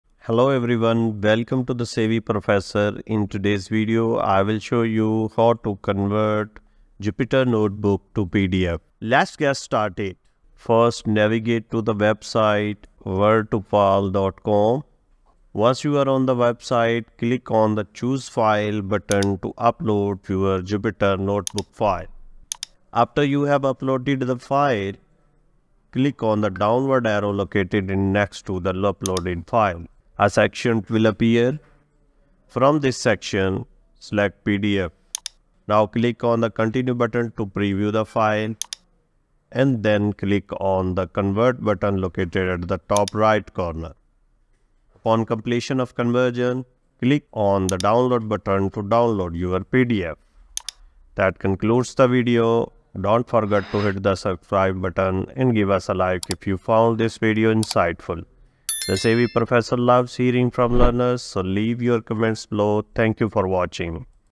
How to Convert Jupyter Notebook to PDF (How to Convert IPYNB to PDF)
How to Convert Jupyter Notebook to PDF (How to Convert IPYNB to PDF). In this tutorial, you will learn how to convert Jupyter Notebook to PDF.